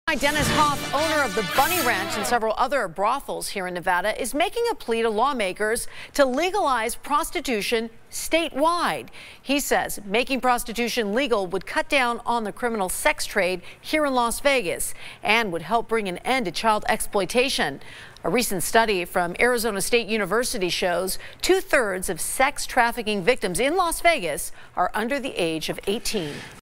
Dennis Hof pushes for legalized prostitution across Nevada
Bunny Ranch owner Dennis Hof says making prostitution legal would cut down on the criminal sex trade in Las Vegas and would bring an end to child exploitation.